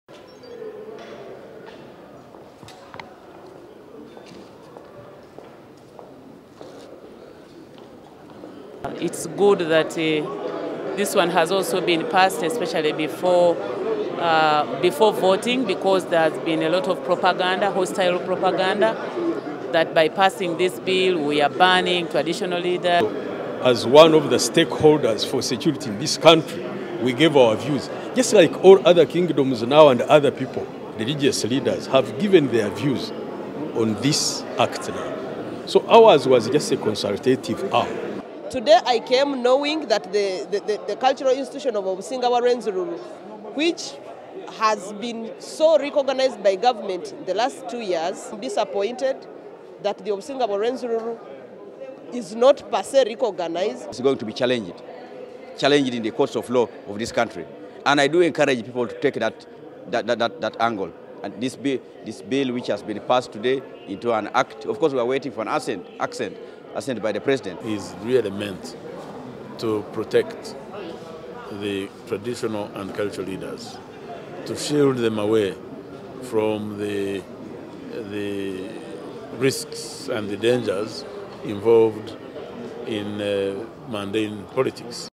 Reactions to Cultural leaders law
Reactions to the passage of the bill was mixed, with opposition leaders vowing to challenge the proposed new law.